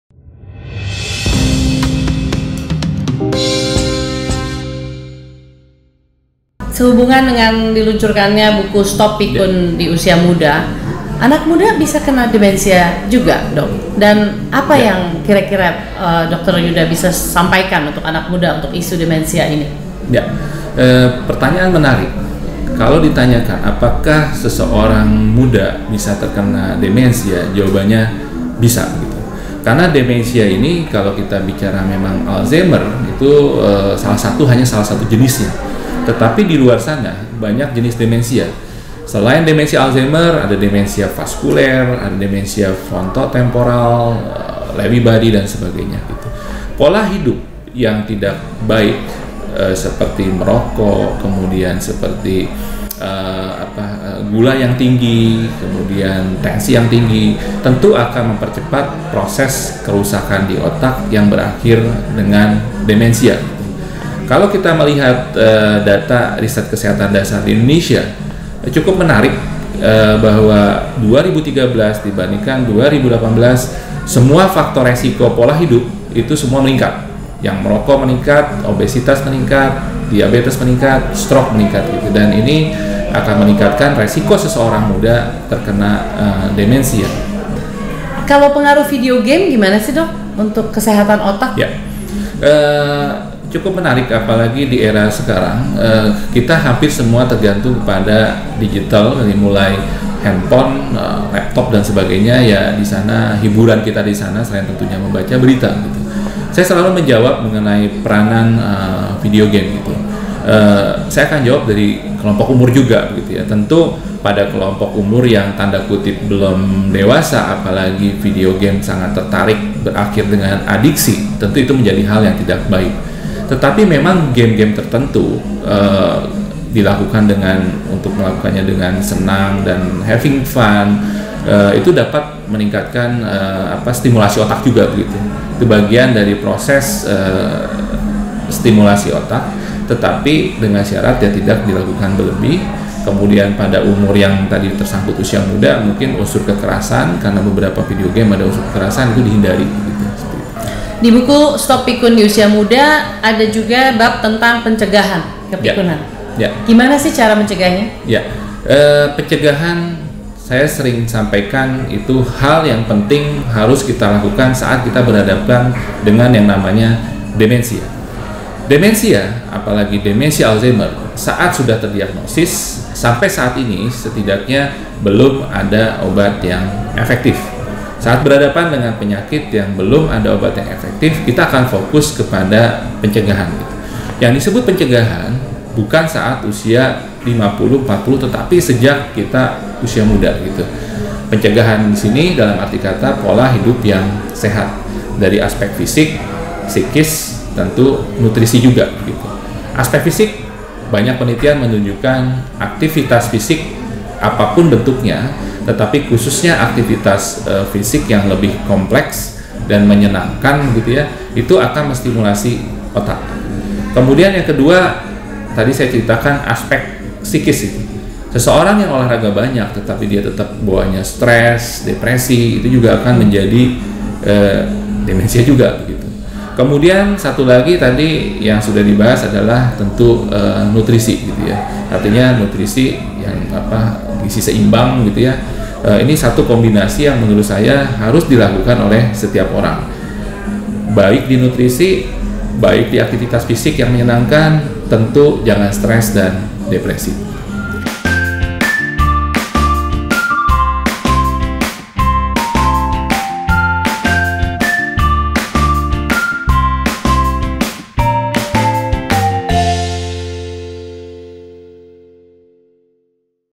Tanya Jawab Dengan DR dr Yuda Turana: Anak Muda Juga Bisa Terkena Demensia, Ketahui Penyebabnya!
- Merchandise ALZI: New Item Masker Elphie silahkan kunjungi website ALZI di store.alzi.or.id atau menghubungi hotline ALZI untuk informasi lebih lanjut.